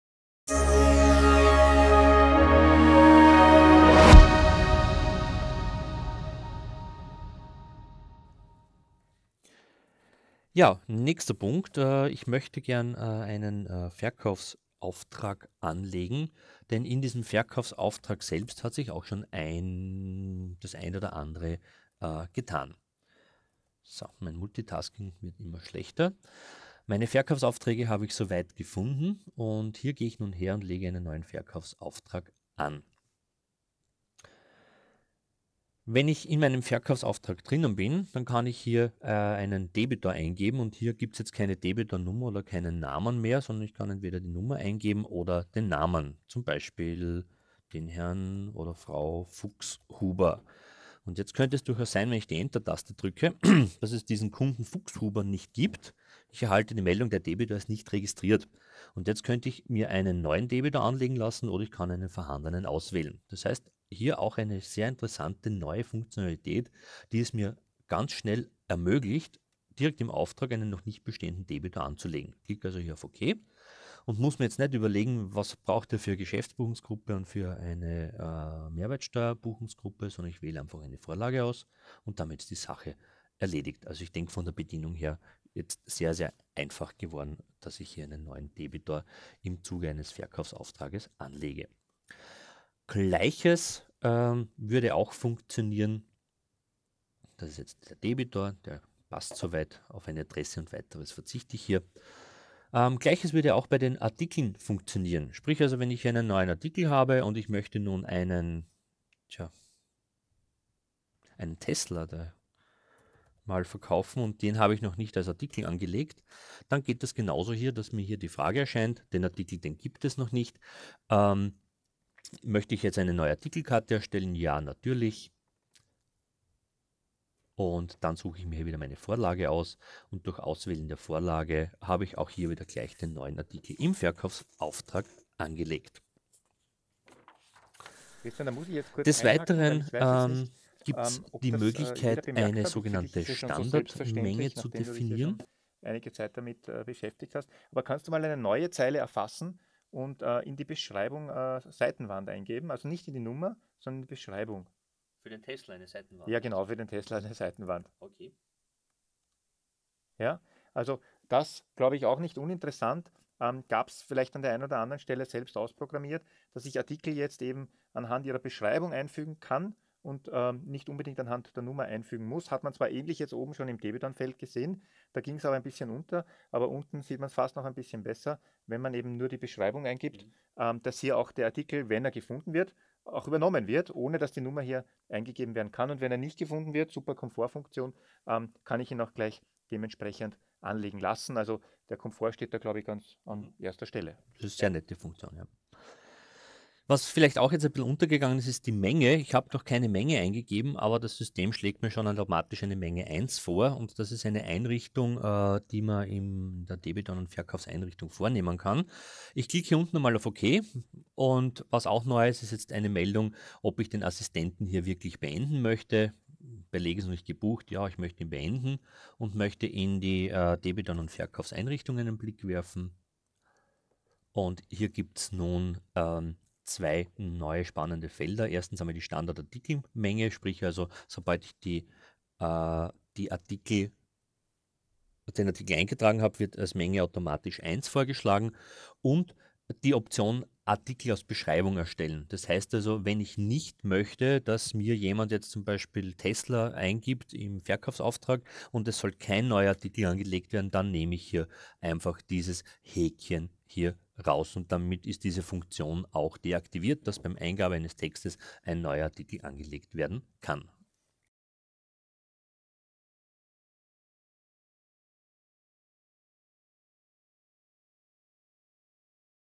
2go NAV 2017 Verkaufsauftrag: Neue Komforfunktionen in der Belegerfassung
In diesem Kurzfilm lernen Sie neue Komfortfunktionen in der Belegerfassung von Dynamics NAV 2017.

Dieses Video ist ein Auszug aus unserer Sendung im Oktober 2016 zu NAV 2017 Neuerungen.